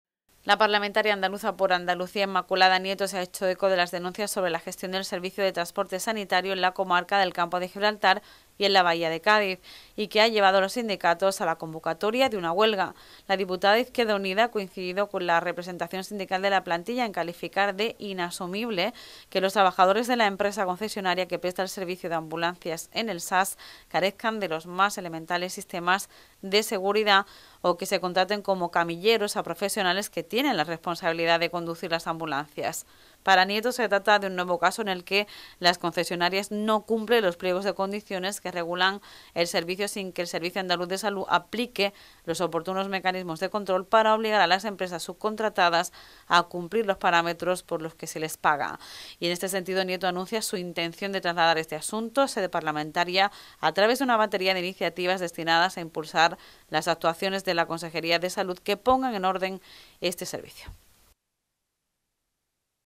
Nieto lleva al Parlamento las denuncias de los trabajadores de las ambulancias
La portavoz del grupo parlamentario Por Andalucía y diputada de Izquierda Unida, Inmaculada Nieto, ha manifestado su solidaridad con la plantilla de la empresa concesionaria del servicio de ambulancias del SAS, que anuncia la huelga a partir de este próximo miércoles en la comarca. Nieto llevará al Parlamento andaluz las reivindicaciones de estos trabajadores.